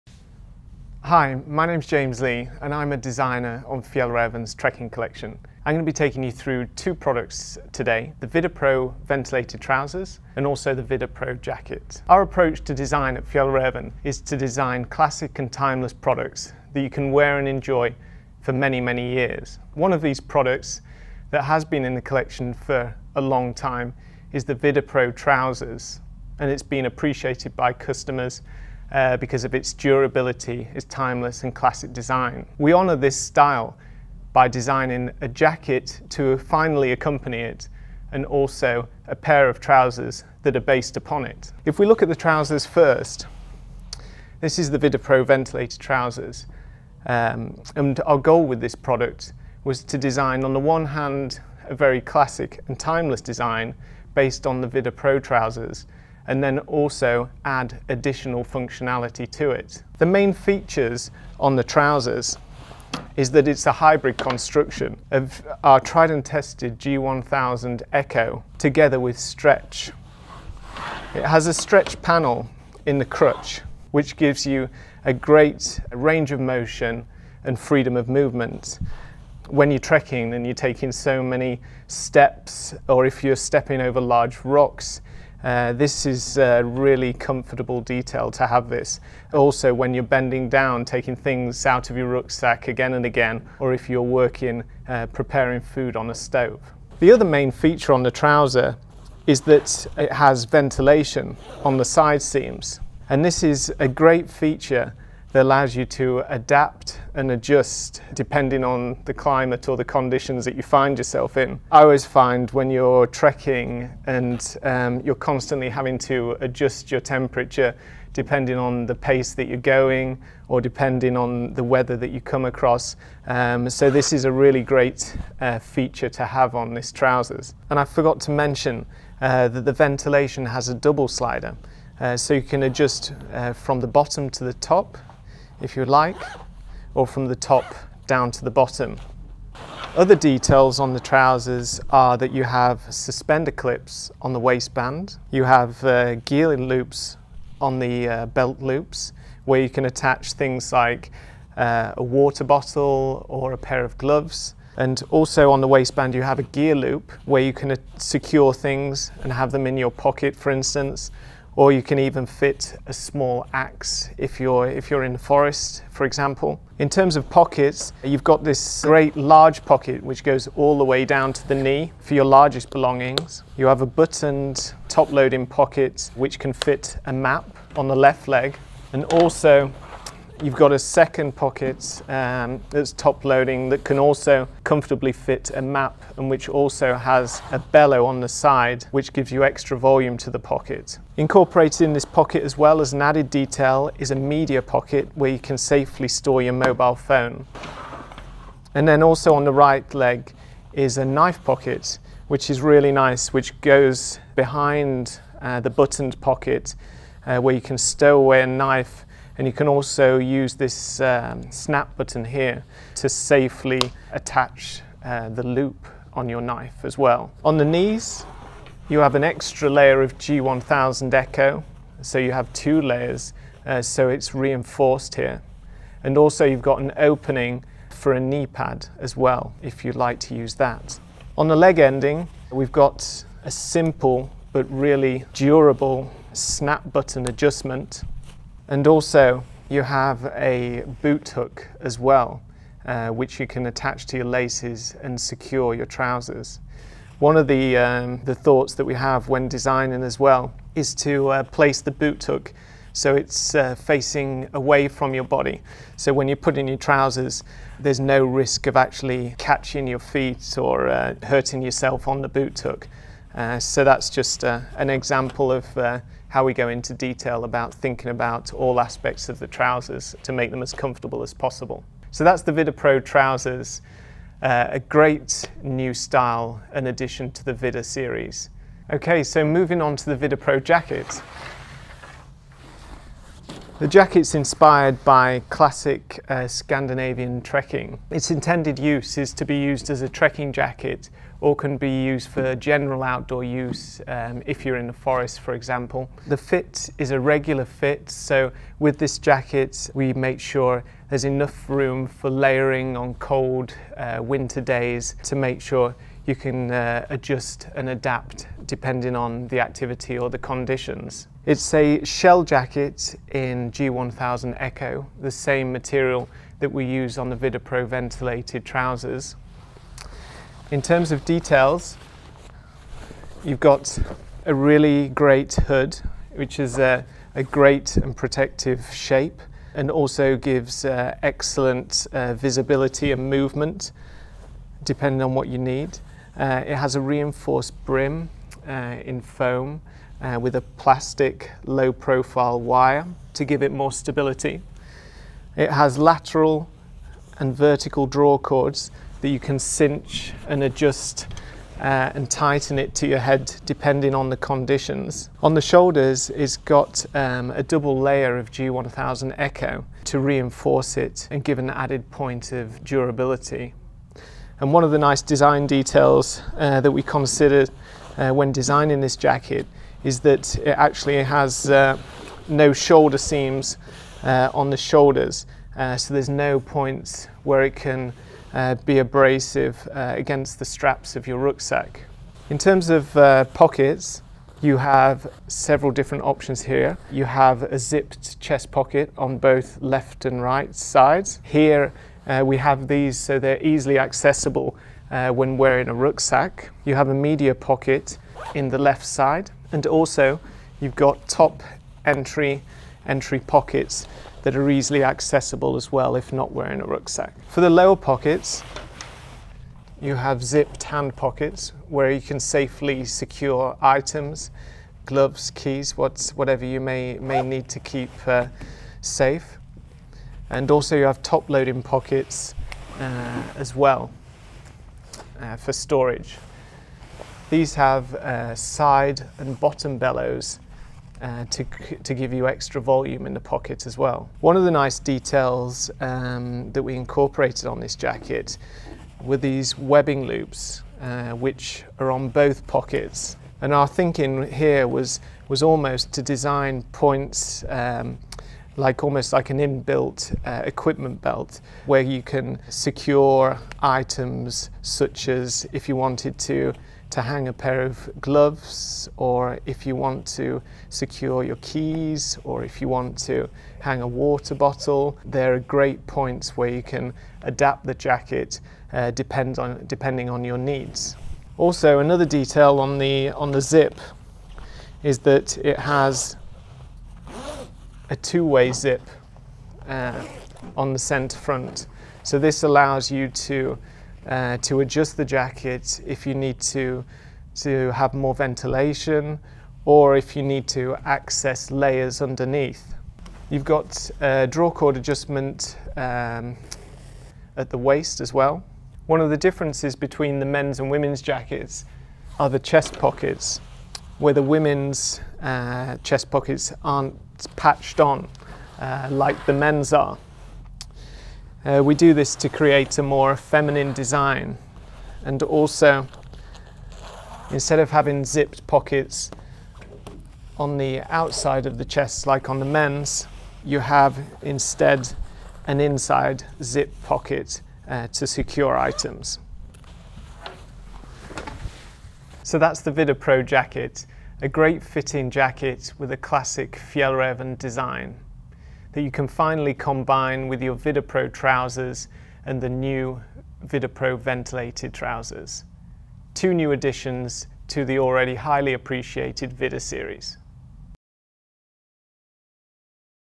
Vidda series | Designer's vision | Fjällräven
This video explains the designer's vision behind Vidda products.